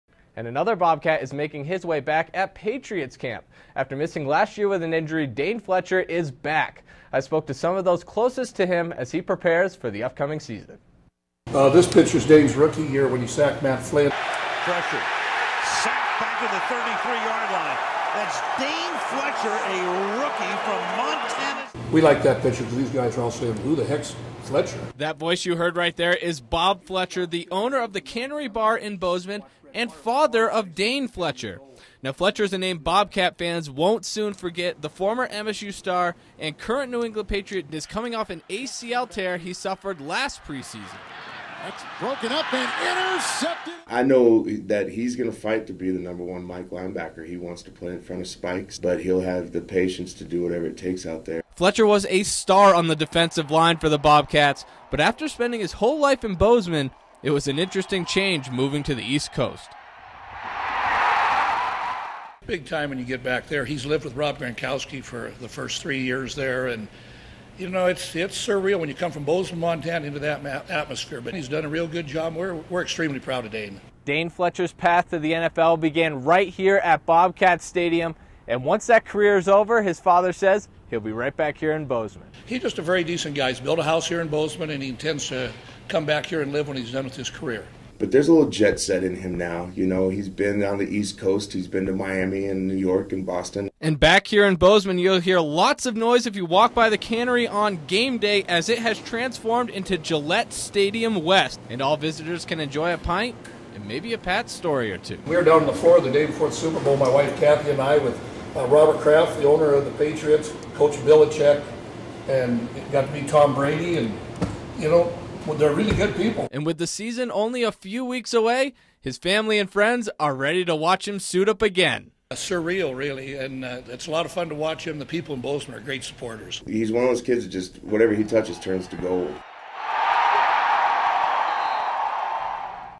Family and friends hope for big NFL season for former Bobcat Dane Fletcher
Package I put together on former Montana State Bobcat and New England Patriot Dane Fletcher, talking to those closest to him as he prepares for season after tearing his ACL last preseason.